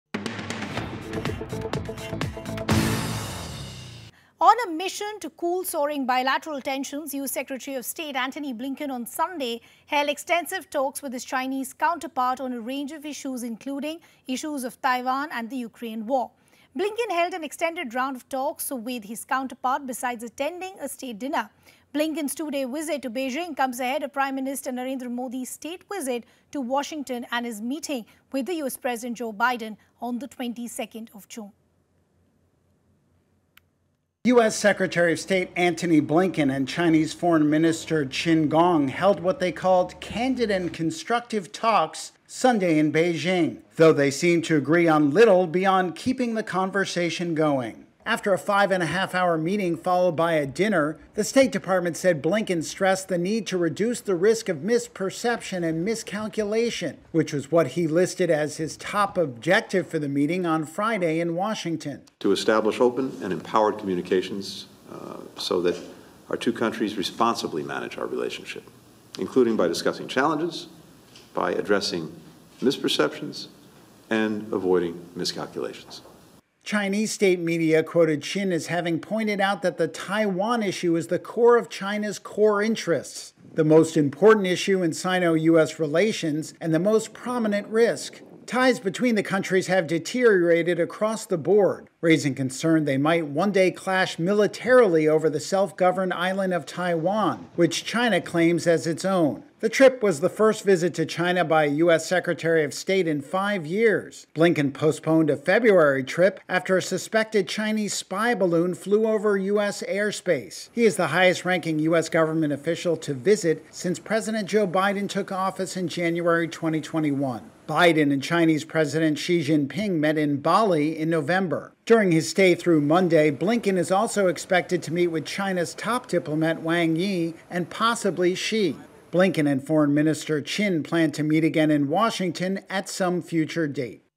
Blinken China Visit | Blinken Meets China’s Xi Jinping In Final Day Of High-stakes Beijing Visit
Chinese leader Xi Jinping and top American diplomat Antony Blinken met in Beijing on Monday, in a potentially crucial step toward patching US-China ties after they cratered in the wake of a dispute over a Chinese surveillance balloon earlier this year.

Blinken is the first US Secretary of State to visit Beijing in five years and his talks with senior Chinese officials are seen as a key litmus test for whether some sort of detente can be forged at a time of lingering distrust.

n18oc_news